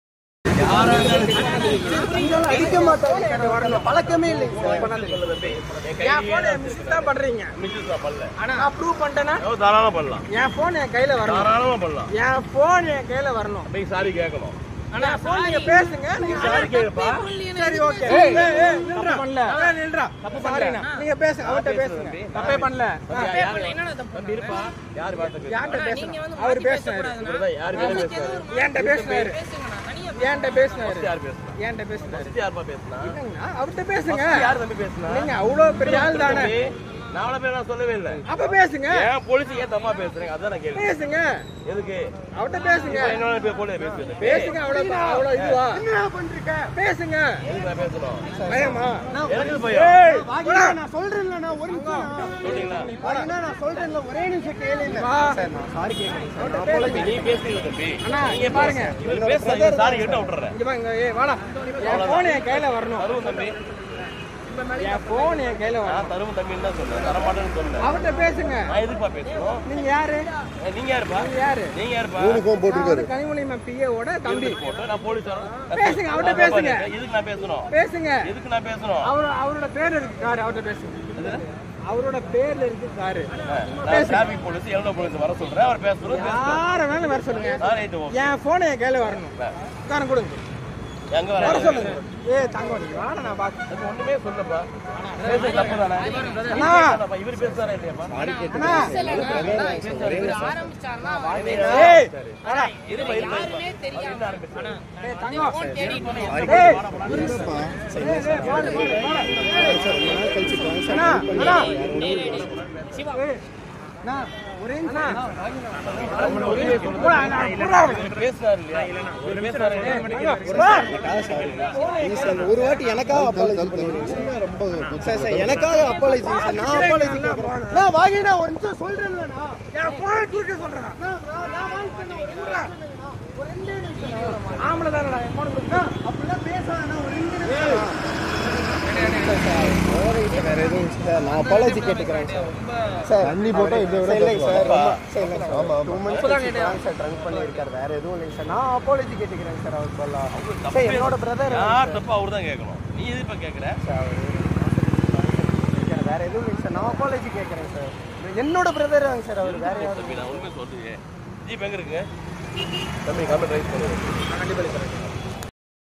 எம்.பி. கனிமொழி உதவியாளரின் தம்பி என்று காவலர்களை மது போதையில் மிரட்டிய இளைஞர் | Tamil News
Coimbatore | கோவை 100 அடி சாலையில் எம்.பி. கனிமொழி உதவியாளரின் தம்பி என்று காவலர்களை மது போதையில் மிரட்டிய இளைஞர்